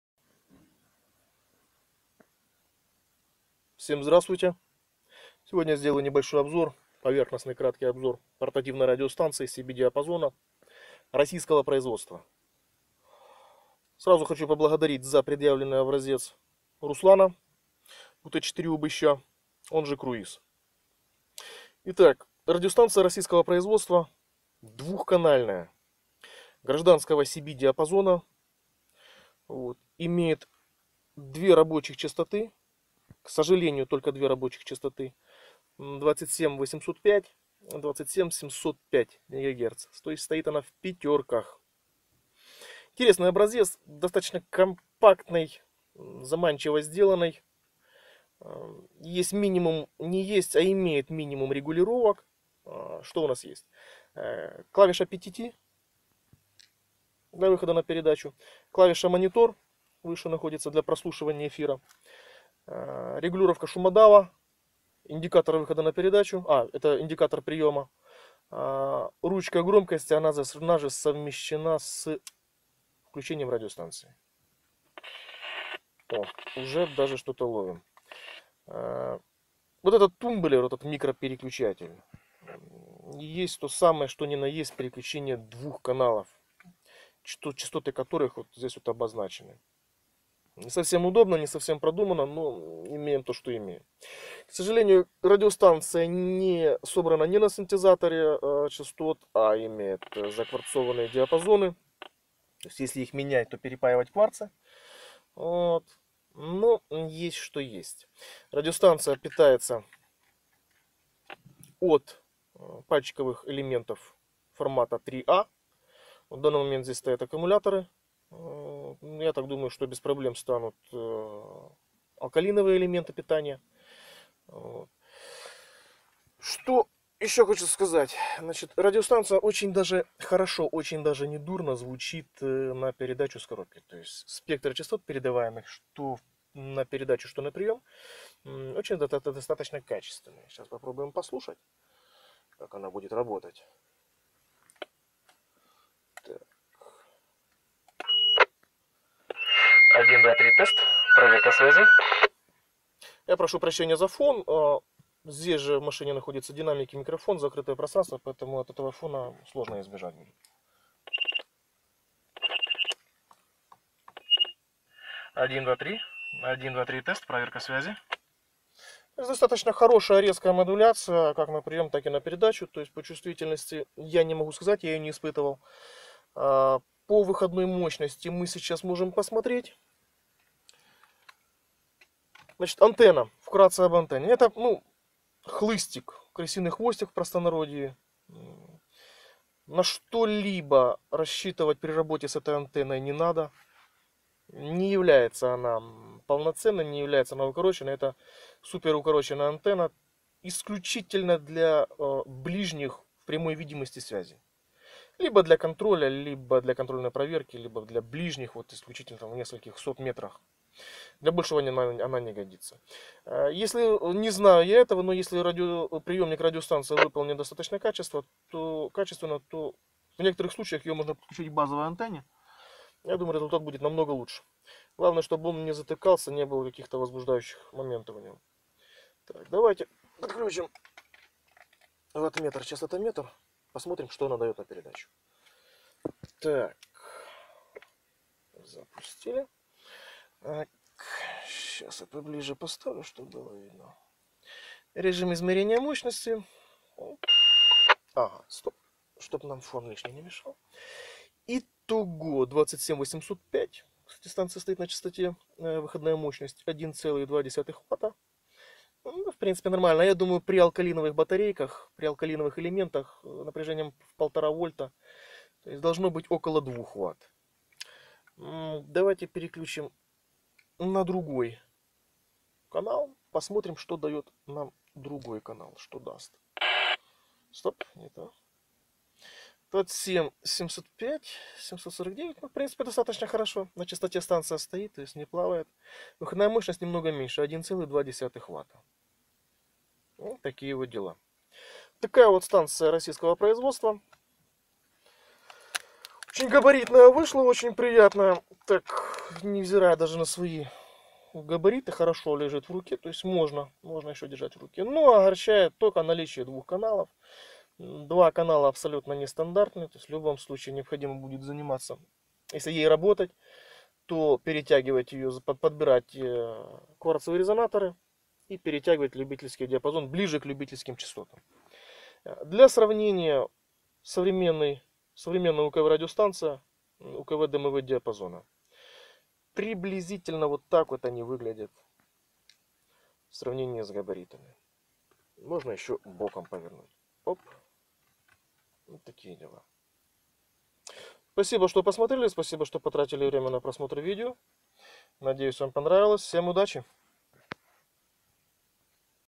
БЕРКУТ-601М2
Портативная ЧМ радиостанция диапазона 27 МГц БЕРКУТ-601М2